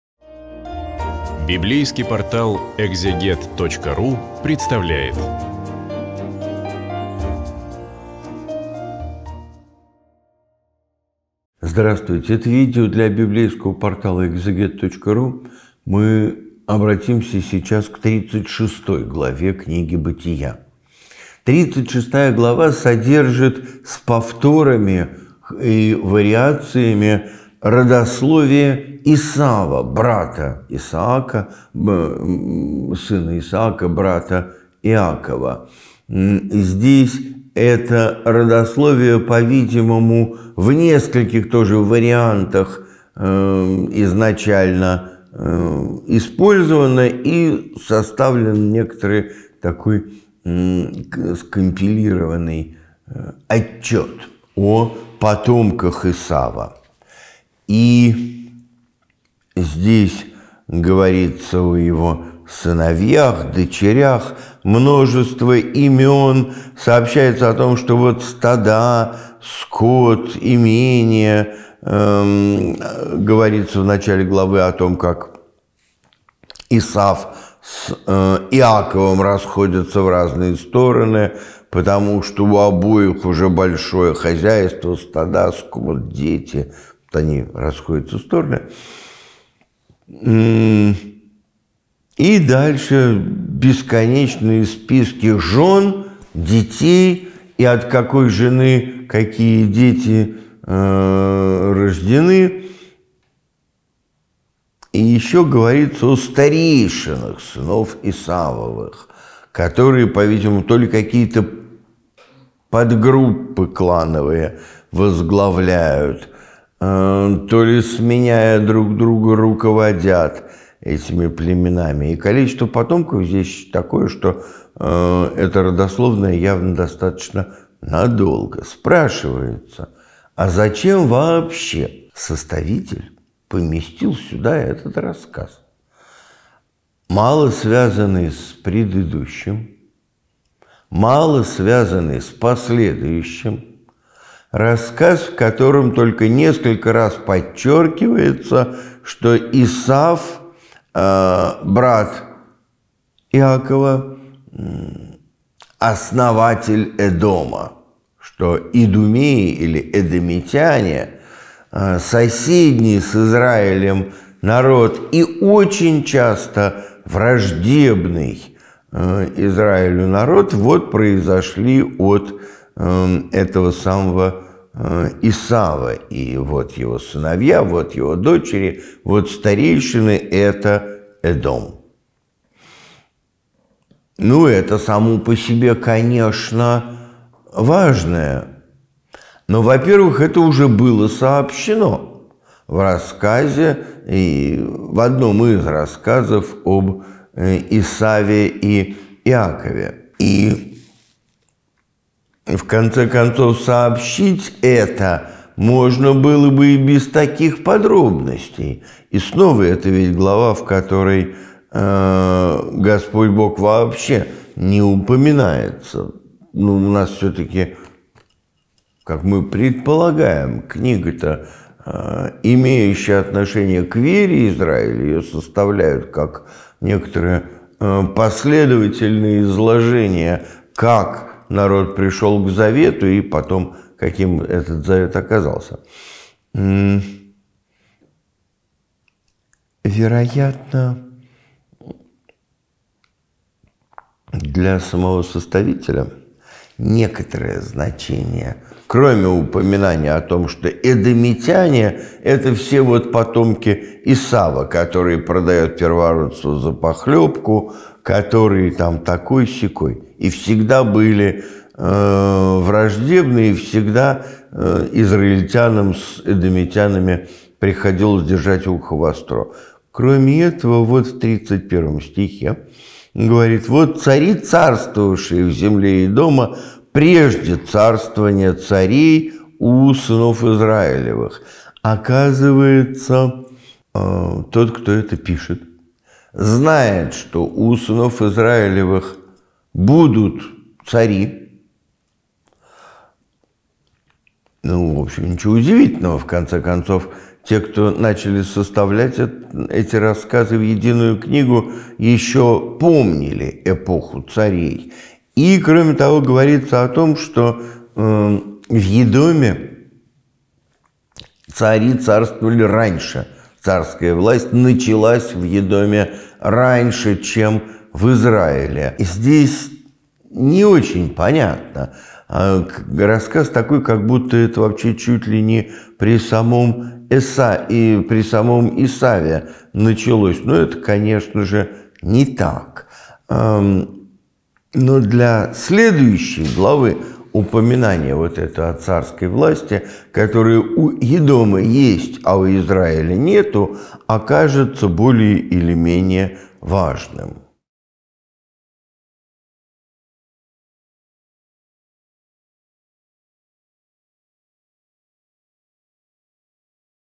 Бытие. Глава 36 "Потомки Исава, старейшины и цари Едома и Сеира" Священник Антоний Лакирев
Команда Экзегет.

Библейский портал Экзегет предлагает цикл лекций священника Антония Лакирева на книгу Бытия. В сегодняшнем видео обратимся к 36-й главе.

🔸  Потомки Исава
🔸  Старейшины Едома
🔸  Цари Идумеи